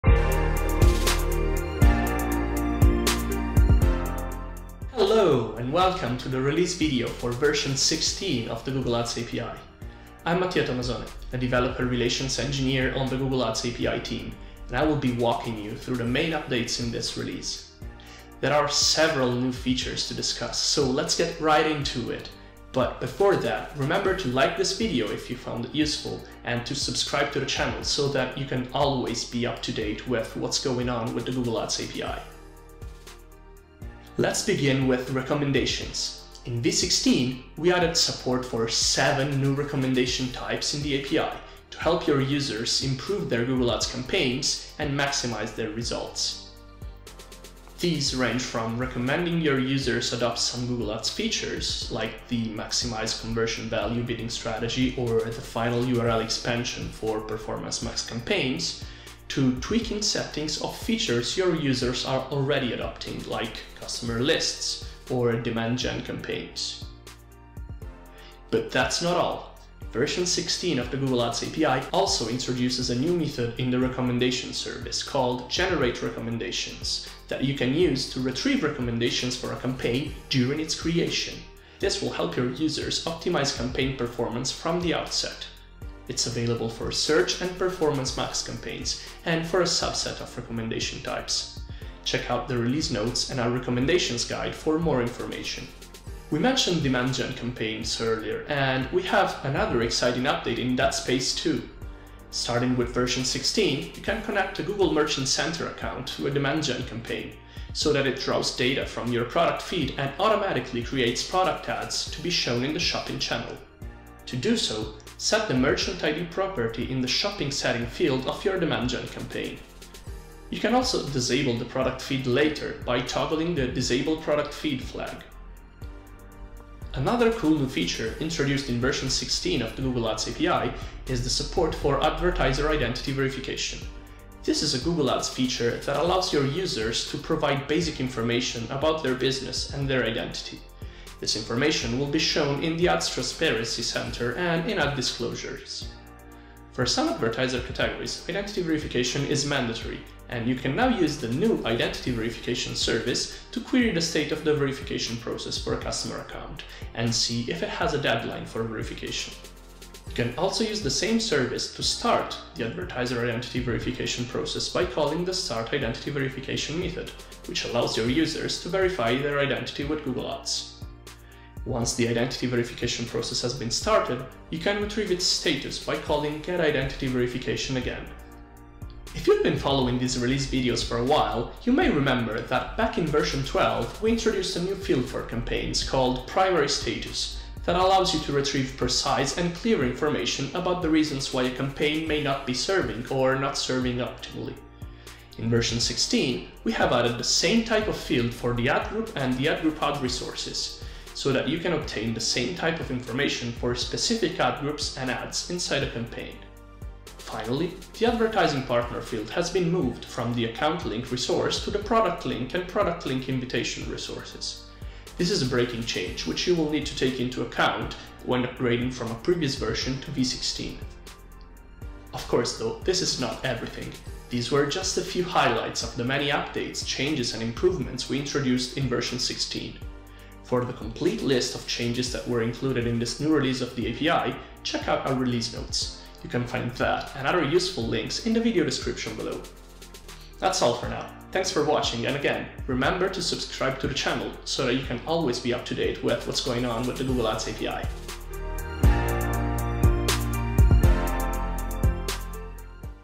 What’s new in Google Ads API v16
Find out about the exciting new features available in the Google Ads API v16.

Chapters:
00:00: Introduction
00:34: Recommendations
01:36: Demand Gen product ads
02:06: Advertiser Identity Verification
02:59: Primary status
03:28: Breaking change: advertising_partner field moved